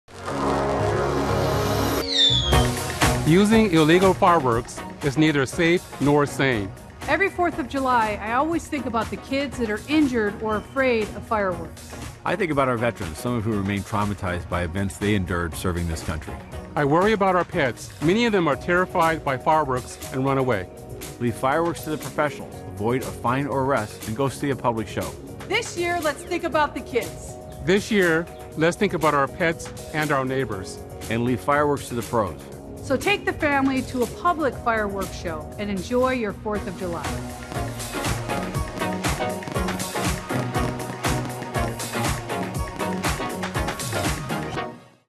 Think of Your Neighbors and Have a Safe4th of July 2022
All fireworks, even the so-called 'safe and sane' variety are illegal in the City of Los Angeles. It may be tempting to acquire personal fireworks for your 4th of July celebration, but this year we ask you to "Think About Your Neighbors." Personal fireworks are known to negatively affect children, pets, and combat veterans. In addition, the wildfire danger is ever present and vegetation moisture levels are significantly drier this year than previously recorded. We encourage all to seek public fireworks shows, which will be more readily available than they were last year. However, those who use, possess, or transport any type of firework may be subject to criminal prosecution - or worse yet, painful and debilitating injury. Surrounding July 4th each year, LAFD responds to several hundred additional emergency incidents than the daily average. We implore you to celebrate safely and enjoy fireworks exclusively at public shows.

This hotline will soon be updated to information relevant to 2022 celebrations.